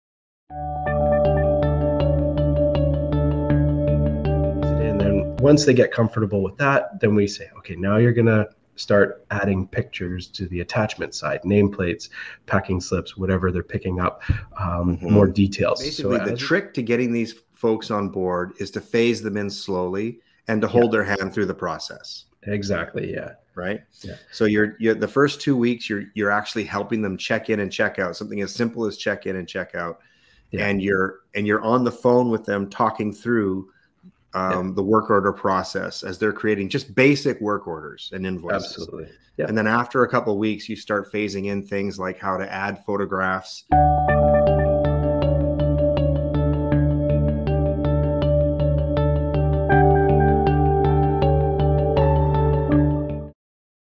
Winning Over Reluctant Techs, Step 3: Adding Pictures (long)
3/6 Ryan Sutherland mentions Step 3 of his process for integrating reluctant techs into the ServiceBox system; teaching them how to attach pictures to work orders (and by extension invoices, job sites, etc.)